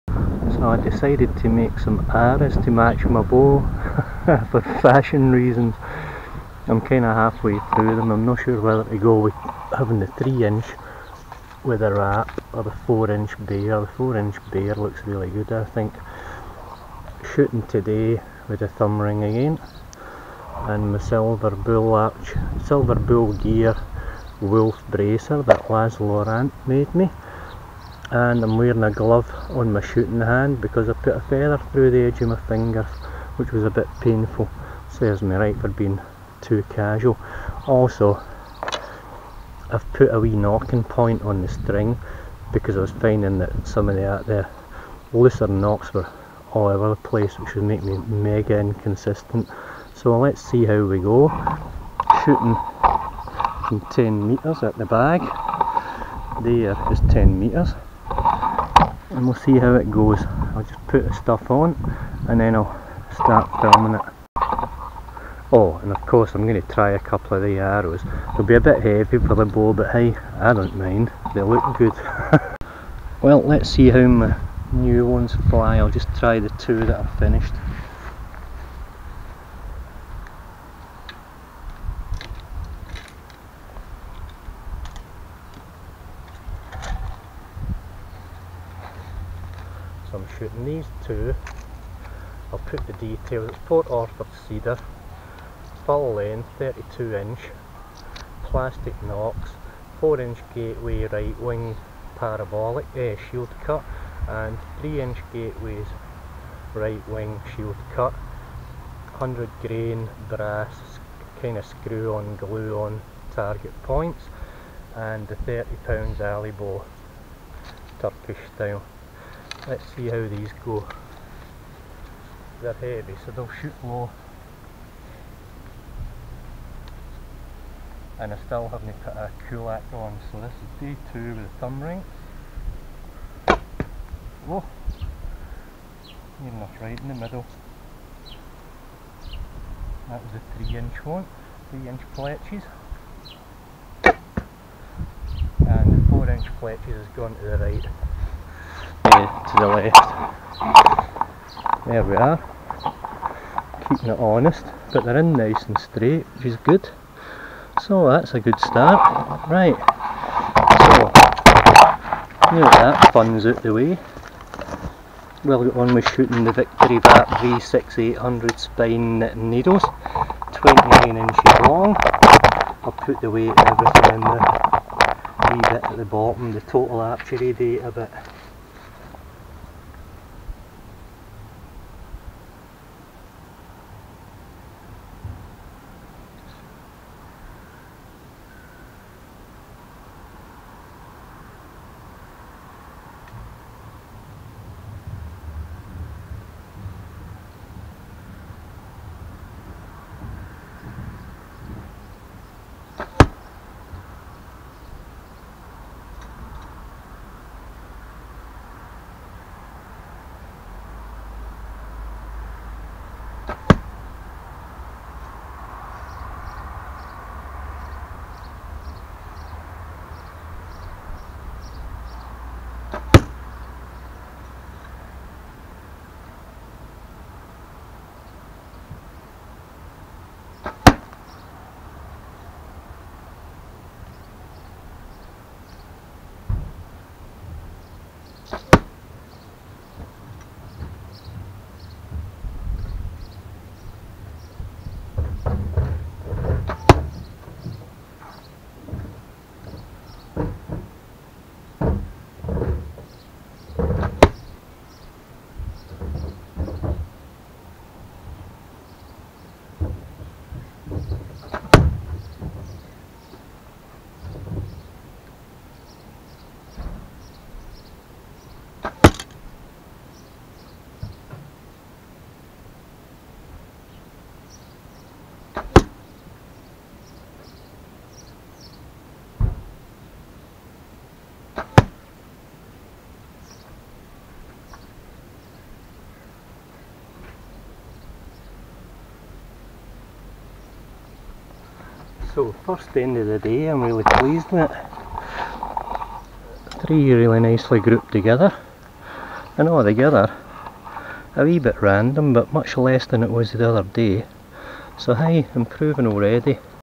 Shooting With A Thumb Ring DAY TWO
The thumbring is too loose on my thumb, and I have yet to make up a kulak to adjust it, which is causing a little inconsistency, but not much.
This is my first end of the day, and a couple of shots with some new wooden arrows I've been making up.